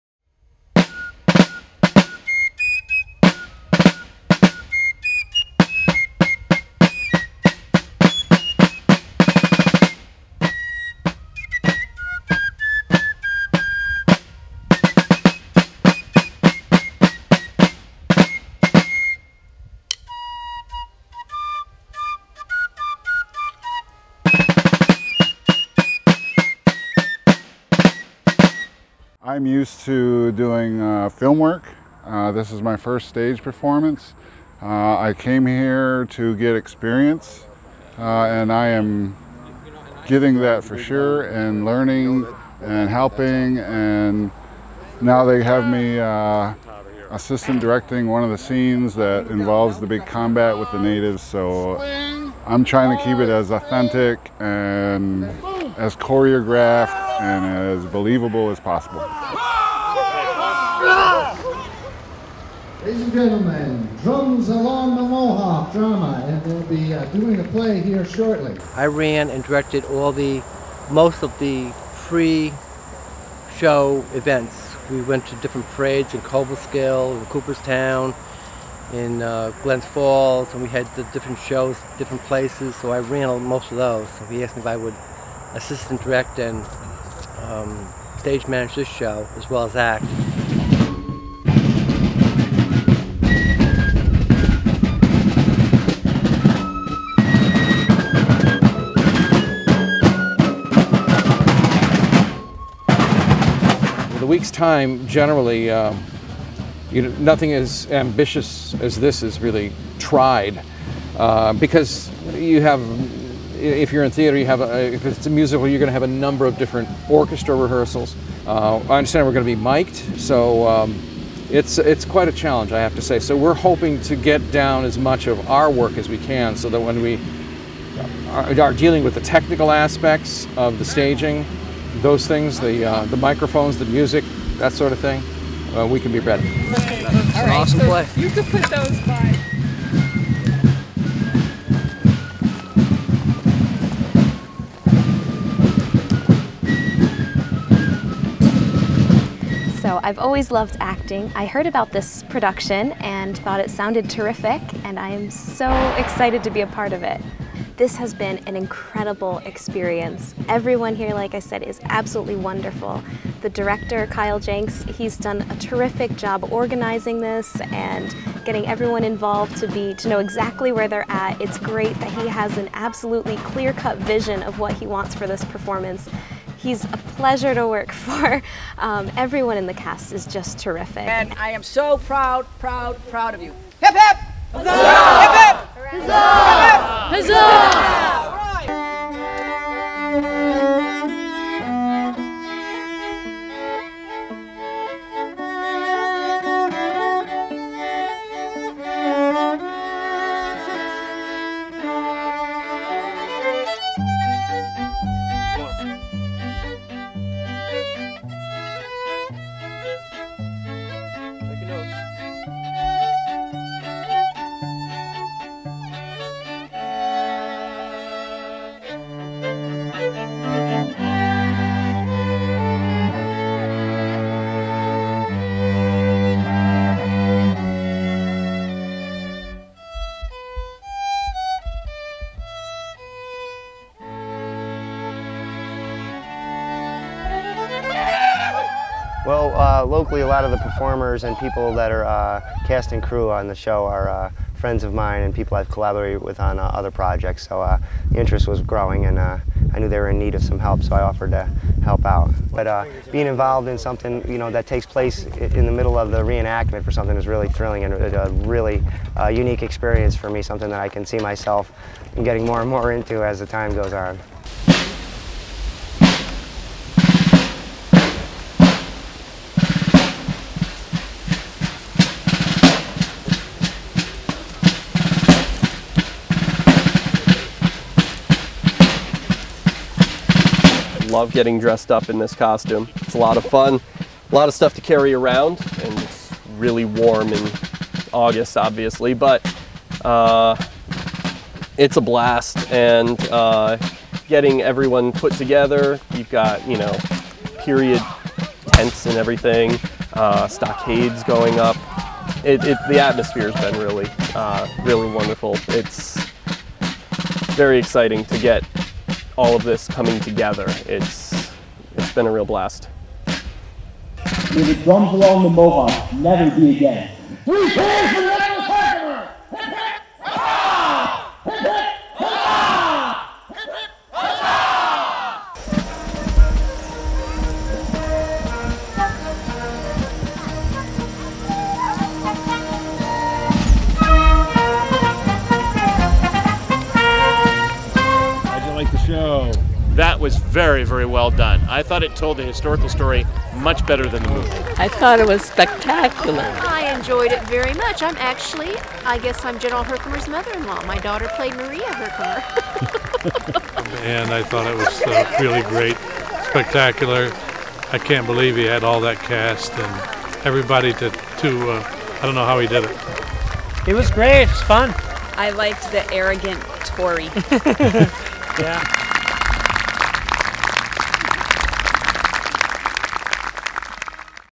Drums Along The Mohawk Outdoor Drama by Walter D. Edmonds
Highlights from Drums Along The Mohawk Outdoor Drama
Performed at Gelston Castle Estate, Mohawk, NY August 4, 2012
Adapted for the outdoor stage by Kyle D. Jenks
Original Score Composed by: Kit Goldstein